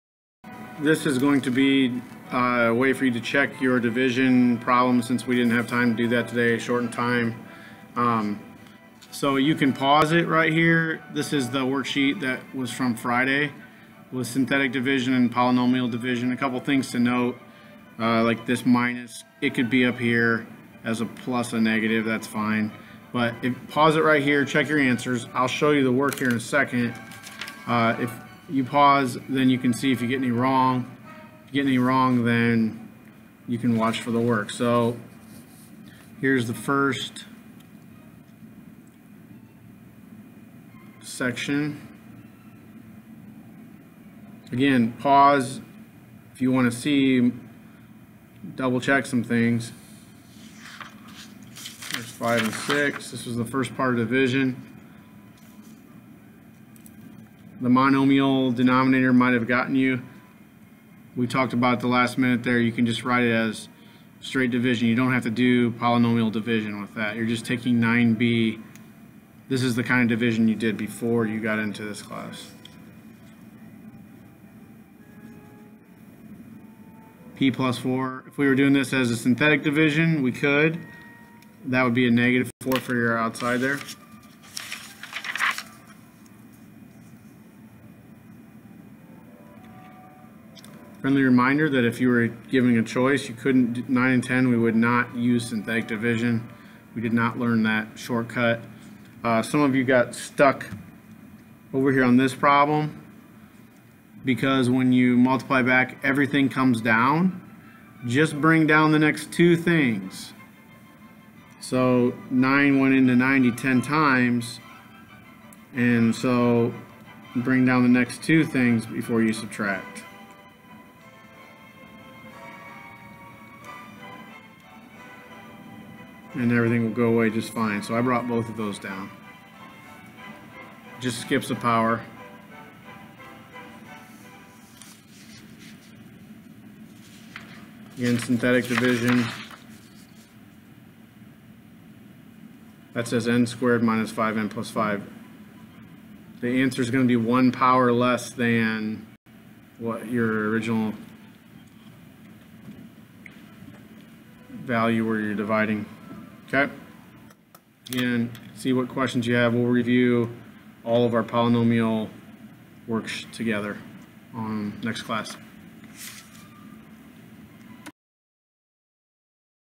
polynomial and synthetic division answer key
Pause to check your answers. Later in the video shows work.